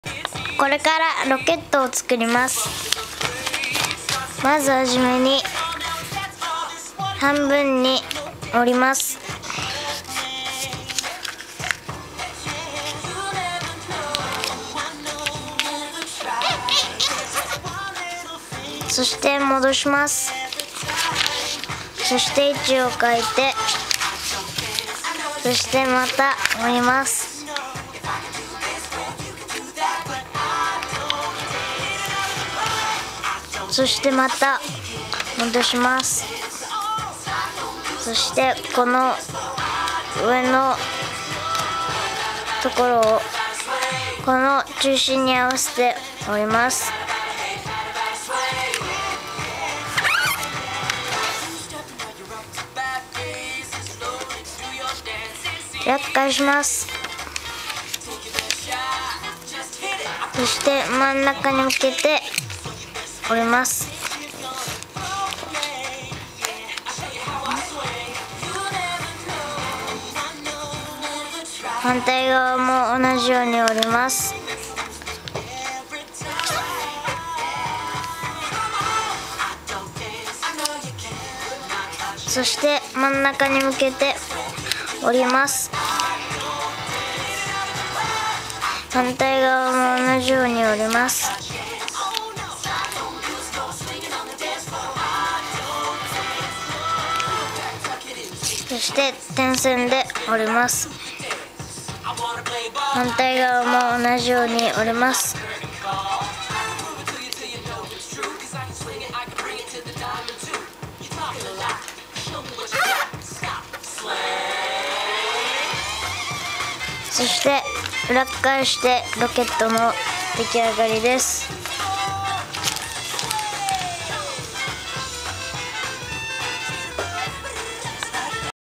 Cohete, rocket en origami
Hecho por mi hijo de 8 años.
Espero que les gusten!